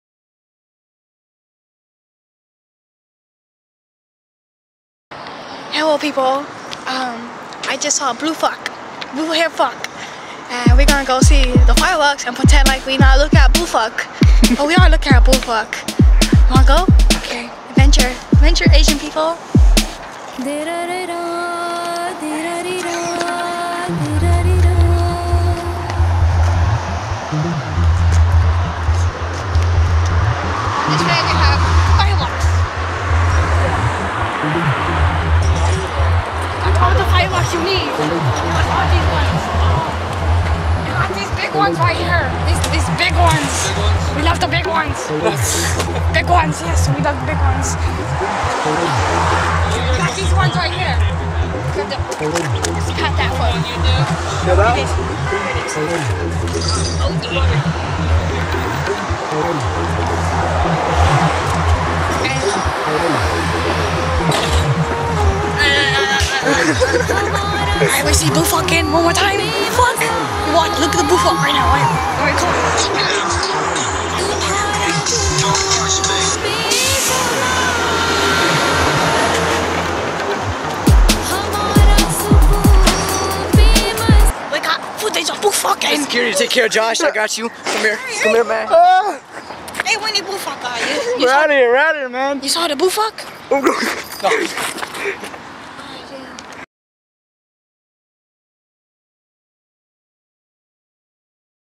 Adventures with asian boy and blue fack!!!!!!
asian boy gets killed by blue fack